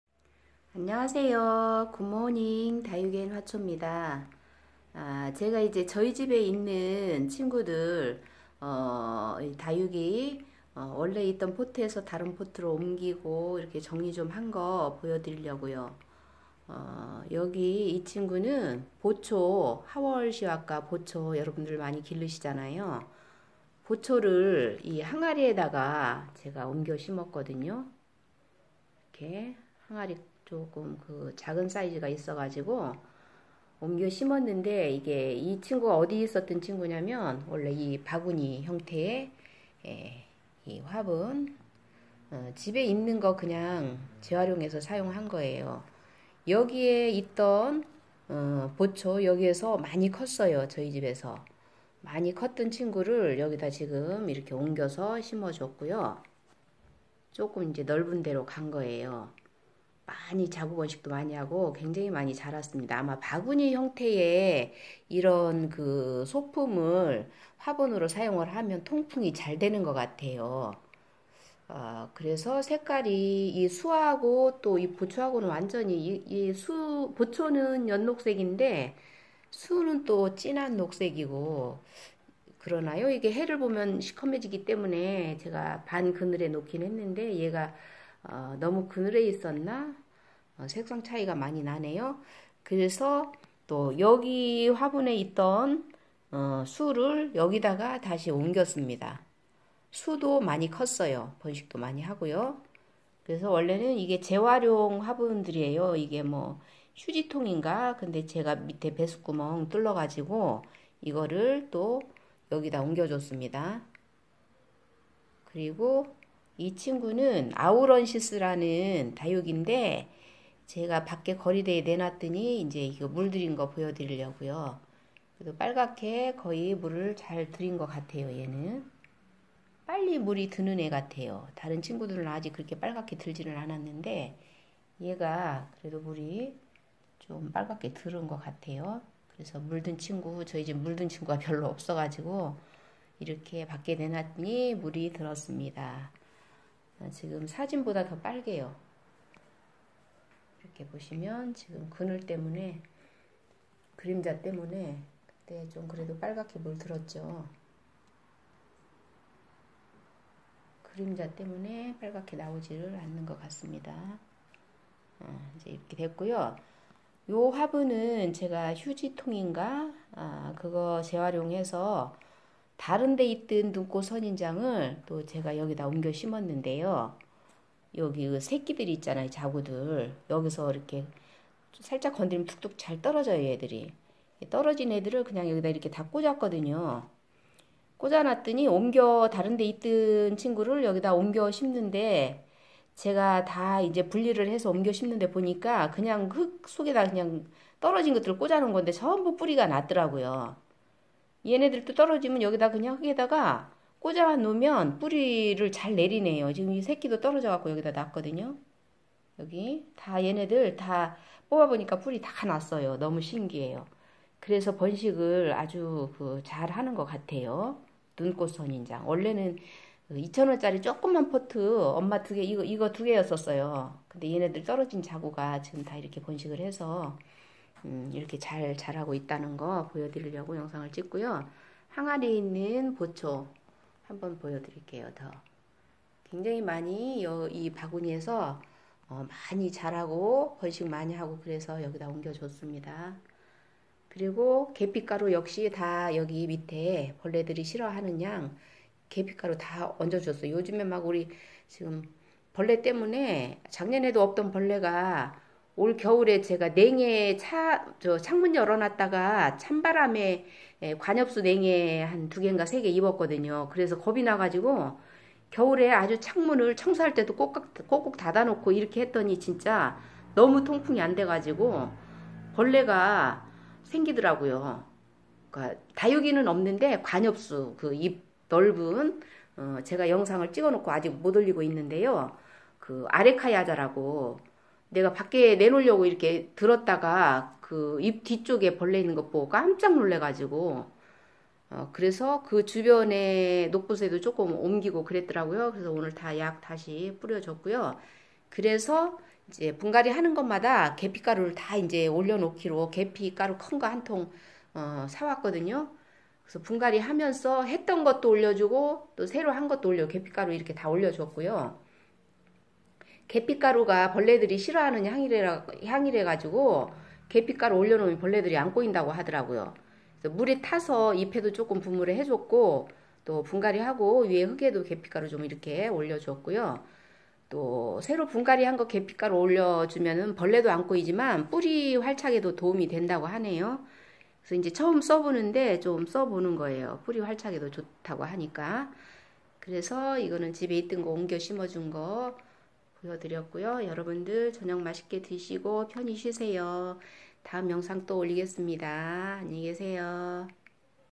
다육이 하월시아 (보초.수.)눈꽃선인장ㆍ아우런시스 예쁘게 성장해 계피가루와 함께 옲겨 줬어요
보초(항아리)수.눈꽃선인장.아우런 시스.많이 성장해서 옮겨 심어 줬어요
계피가루♡뿌리활착에 도움이 되며 벌레피해를 막기위해 화분위에 살짝 뿌려 줬어요